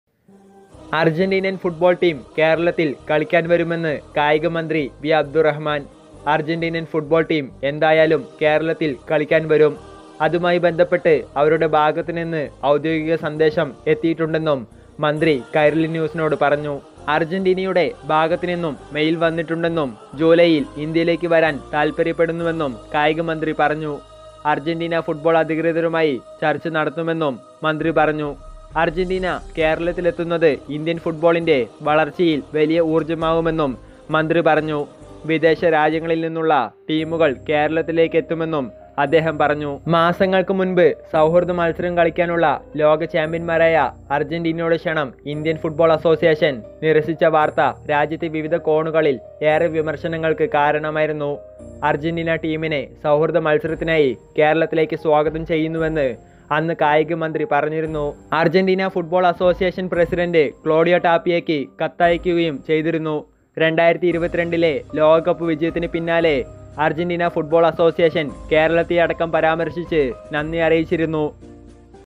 അർജന്റീന ടീം കേരളത്തിൽ കളിക്കാൻ സമ്മതം അറിയിച്ചു എന്ന് കായിക മന്ത്രി | Argentina | Lionel Messi
Lionel Messi
Argentina to kerala
Argentina
Argentina malayalam
Messi malayalam